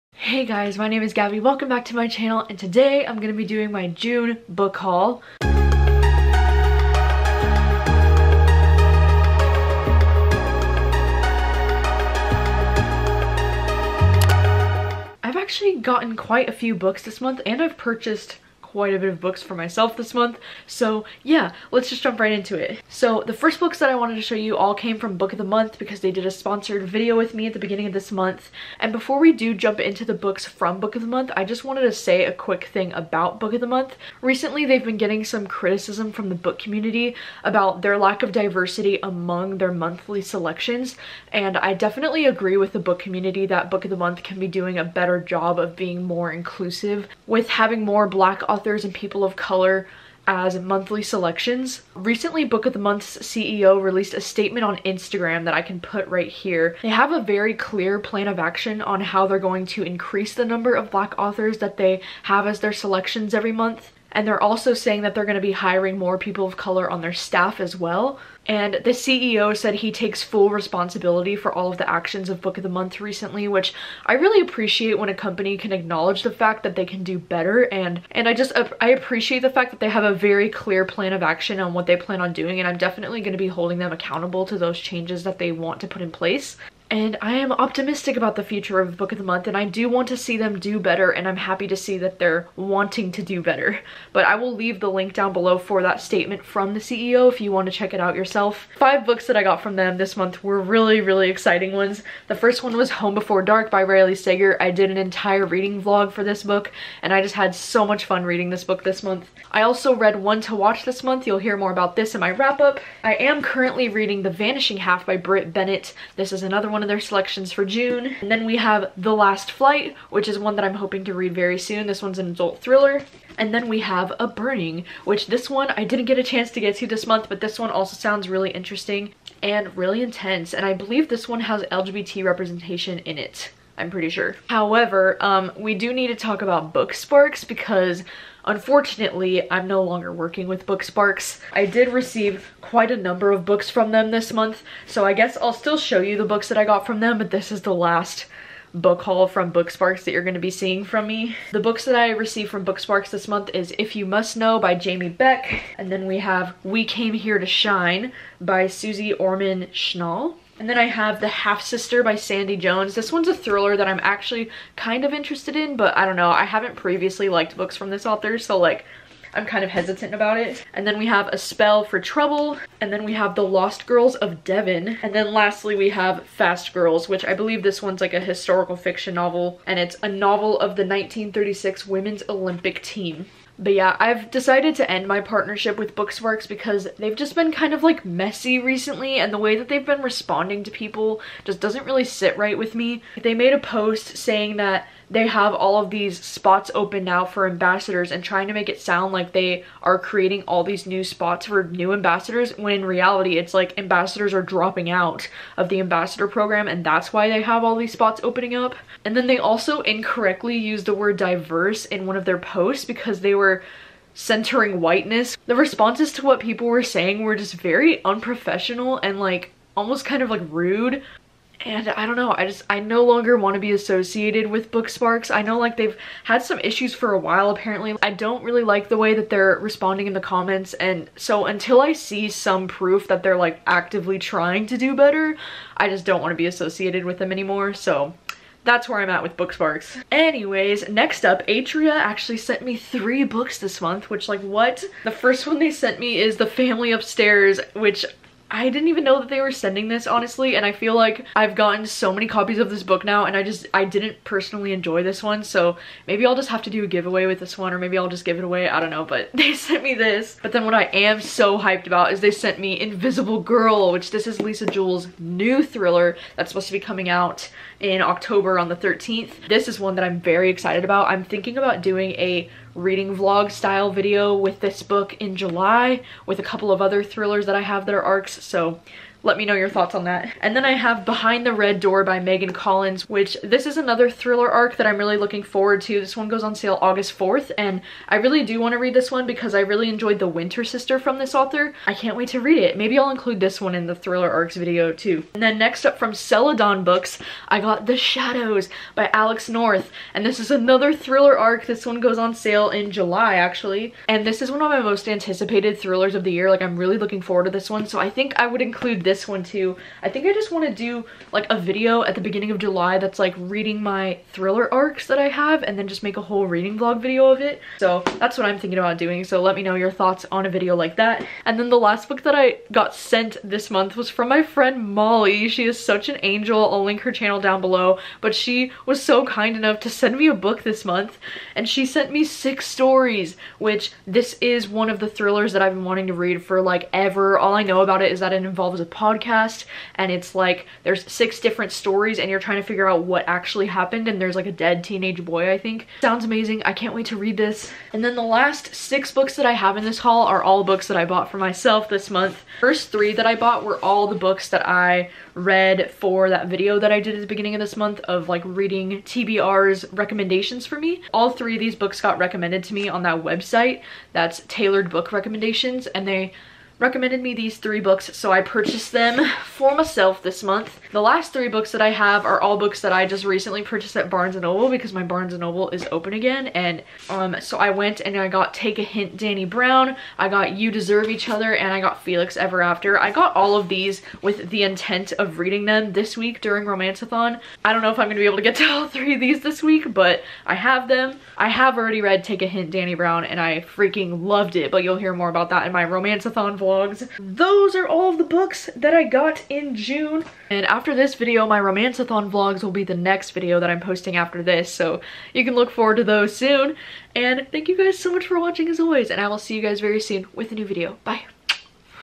June Book Haul
Here's my June bookhaul, let me know how many books you hauled in June and if you've read any of these books let me know your thoughts!

L E T T E R B O X D 🎥